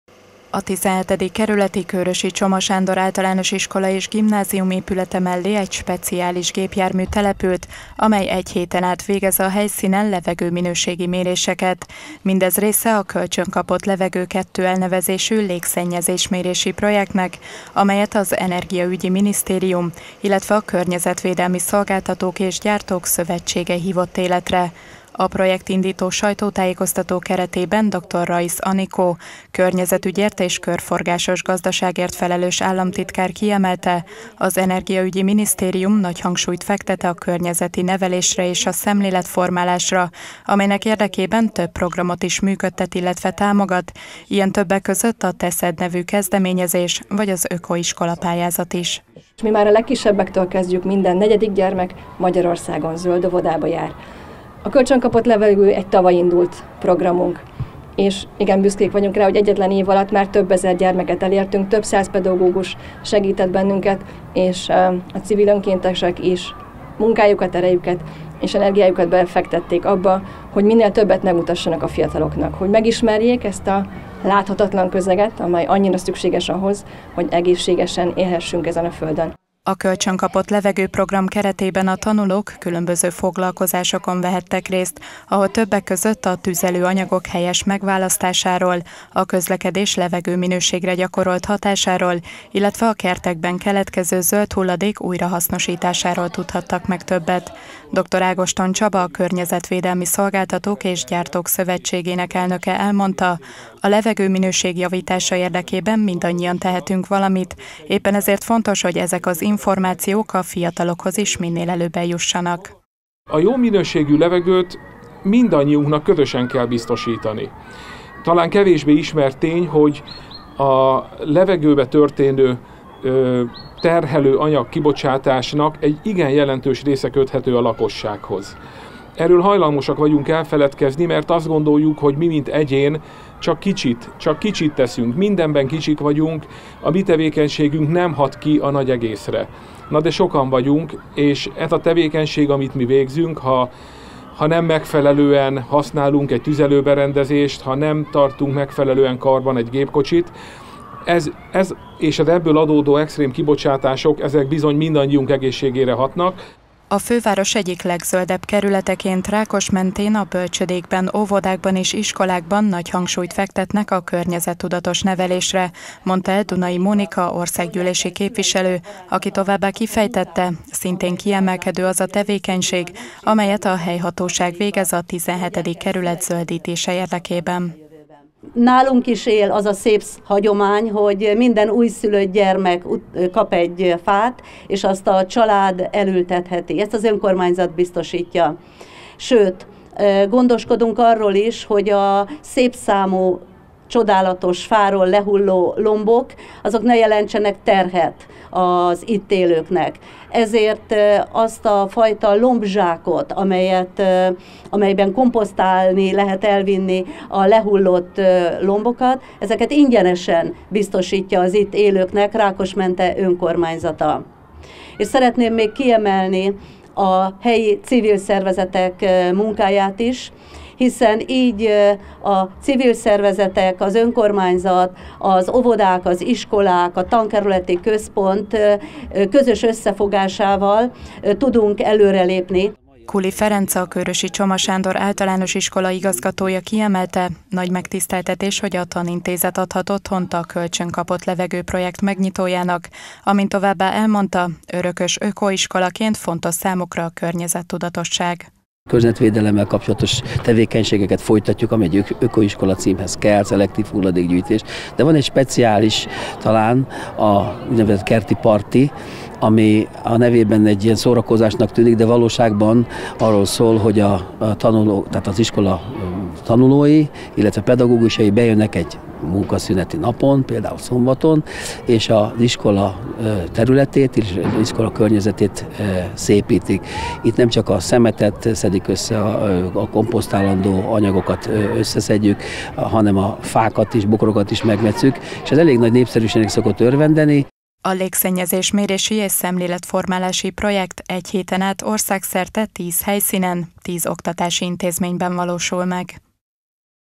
Krónika: Elindult a Kölcsönkapott levegő 2 program (2024.04.22.)
Az Energiaügyi Minisztérium, valamint a Környezetvédelmi Szolgáltatók és Gyártók Szövetsége „Kölcsönkapott levegő 2” elnevezéssel indított légszennyezés mérési, illetve szemléletformálási projektet, melynek idei első állomása a XVII. kerületi Kőrösi Csoma Sándor Általános Iskola és Gimnázium volt.